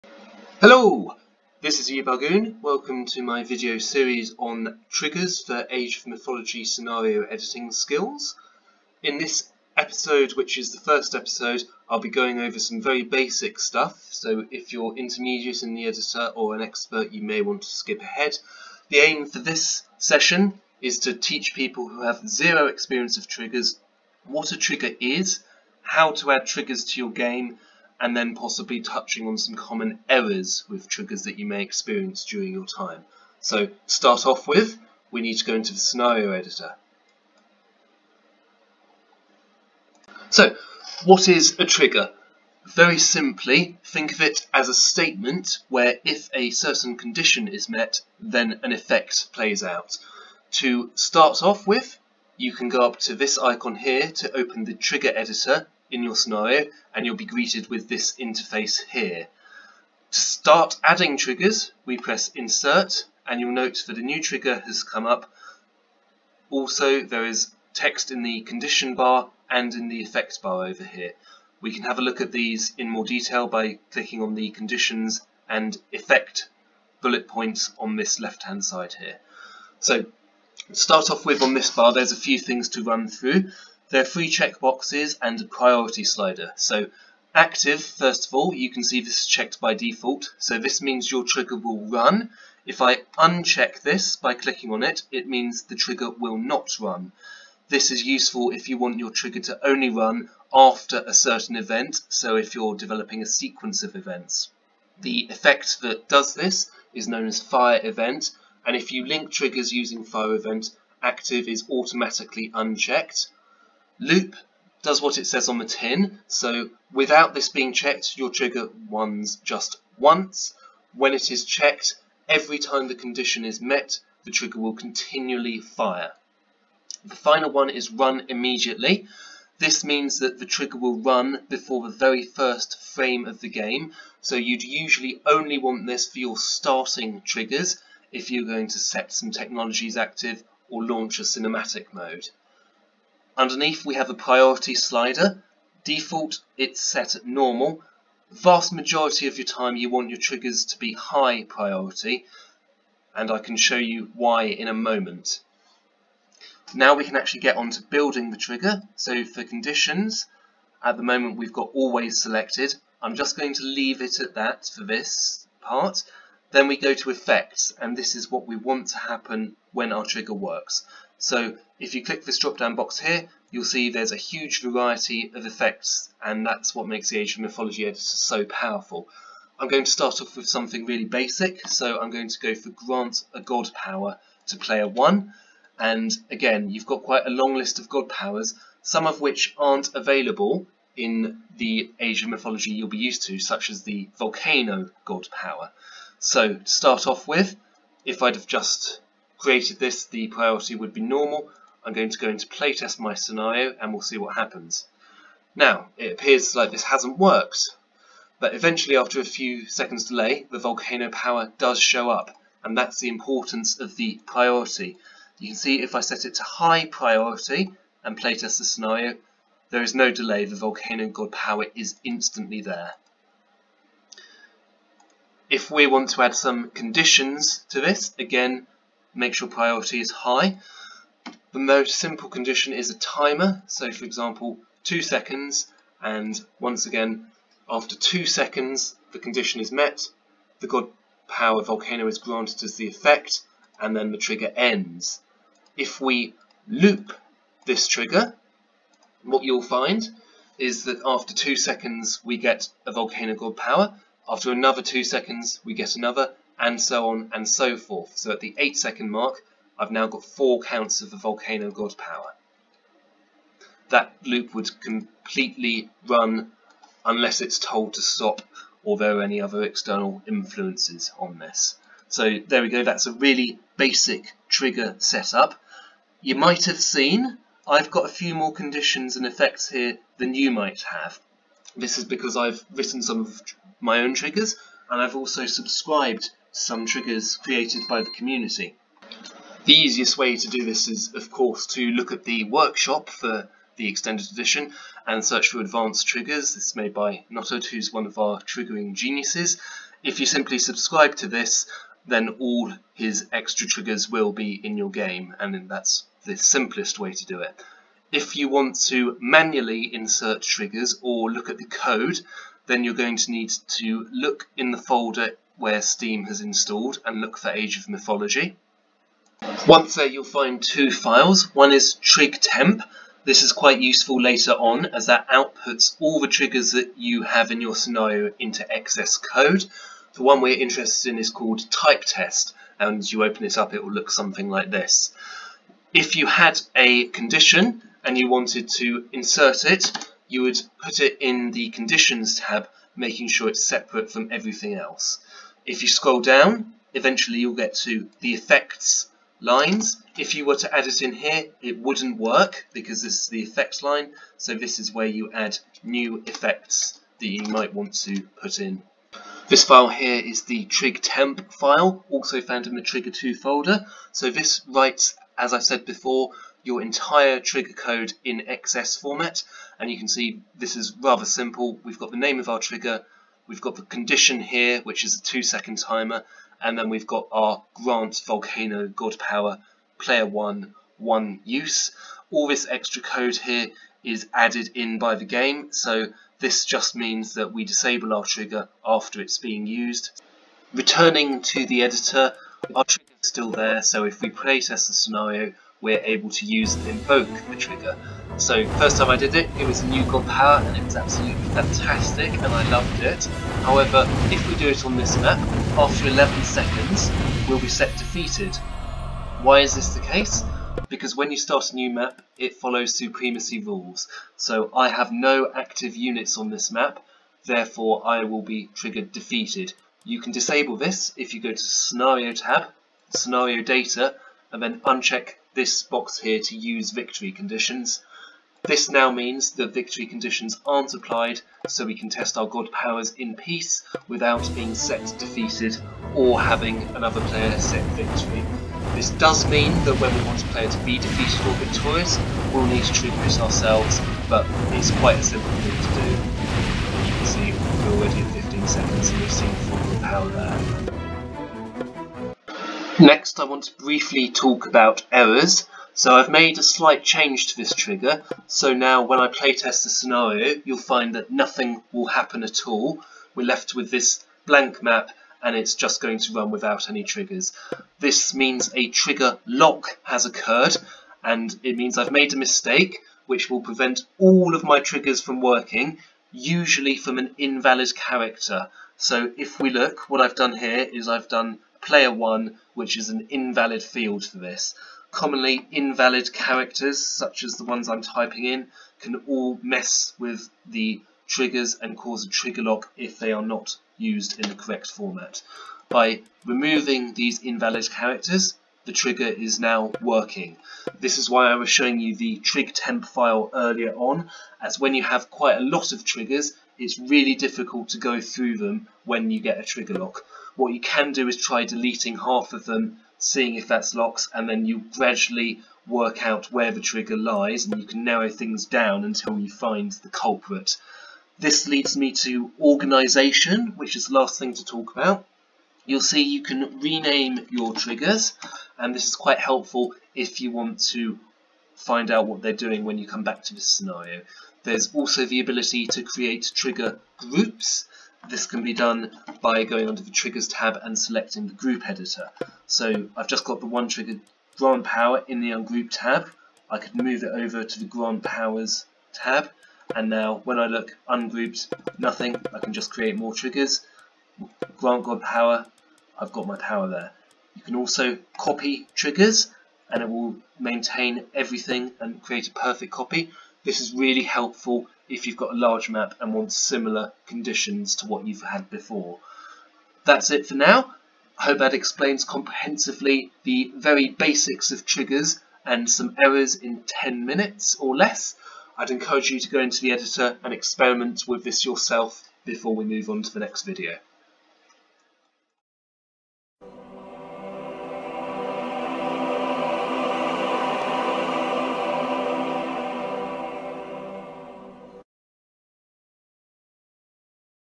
Basics: 01 What is a trigger?
An introduction aimed at those who have never triggered before. What is a trigger? What makes up a trigger? How do we install custom triggers? And what can go wrong?
 [Chapter Content]
0:00 - Intro
0:40 - What is a trigger?
4:49 - Installing custom triggers
6:35 - Volcano god power
6:47 - Victory Conditions
7:32 - Trigger lock/errors
8:42 - Trigger organisation